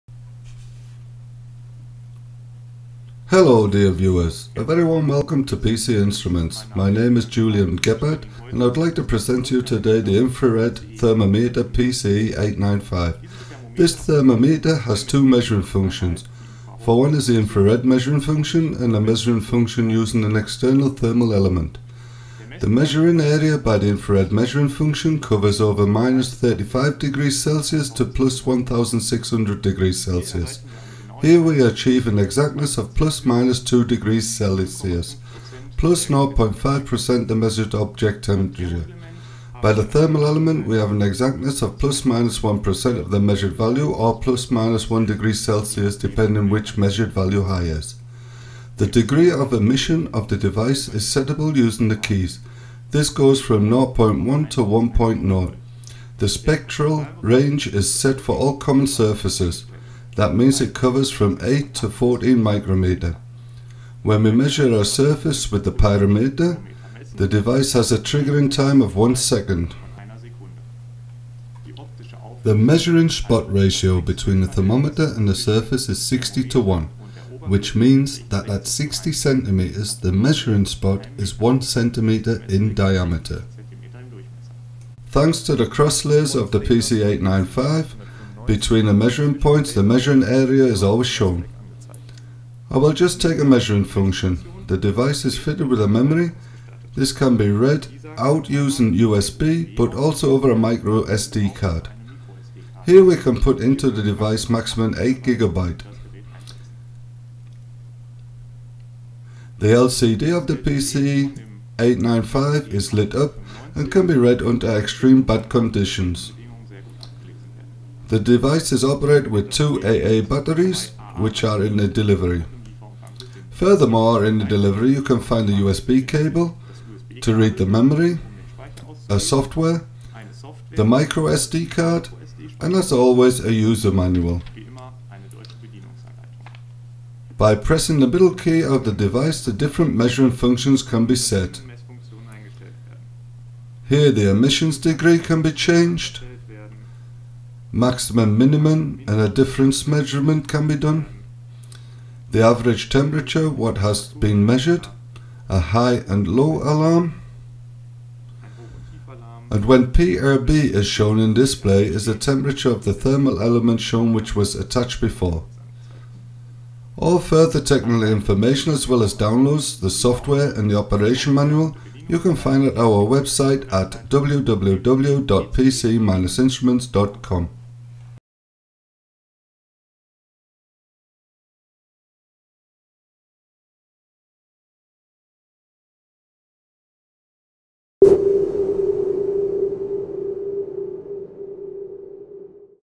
Product Presentation Infrared Thermometer PCE-895
Julian Gebhardt introduces the Infrared Thermometer PCE-895.